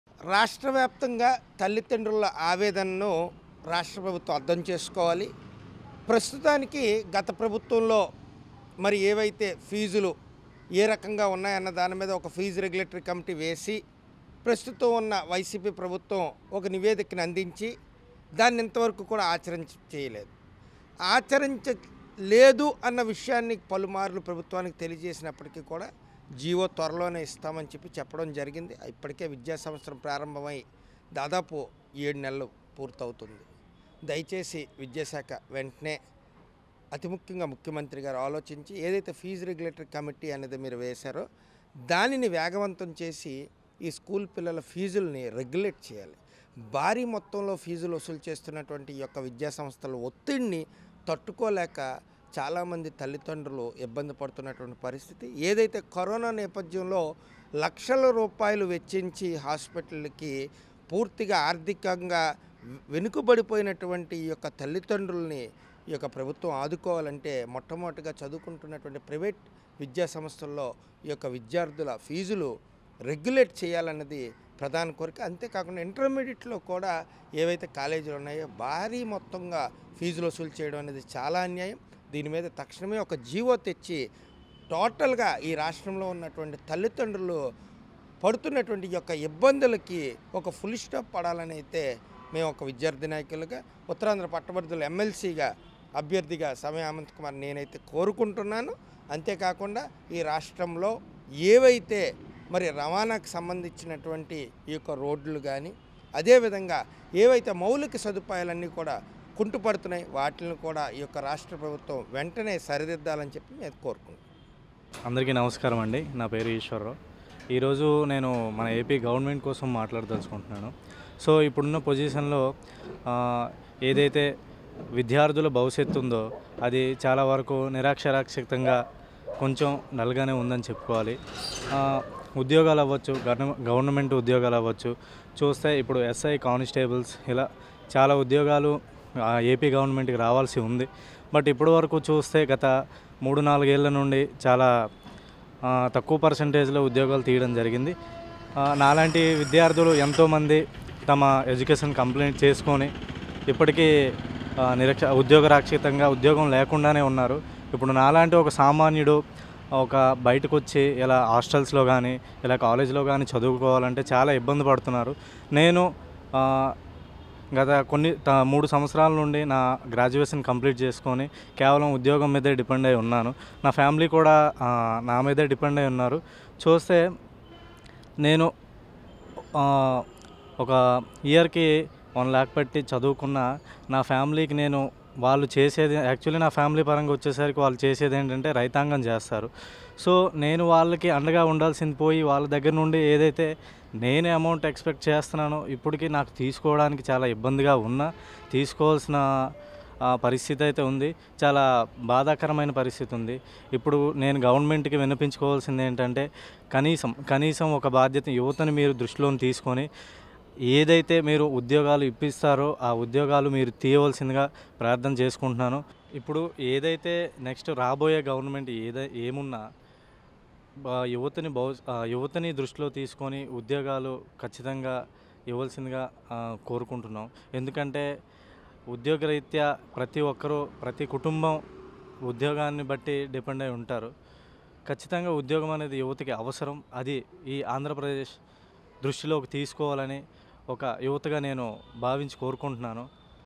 ఈ వీడియో చూసైనా బుద్ధి తెచ్చుకోండి జగన్ | Unemployed Youth On YS Jagan Over Job Calendar | Open Talk
Category
News & Politics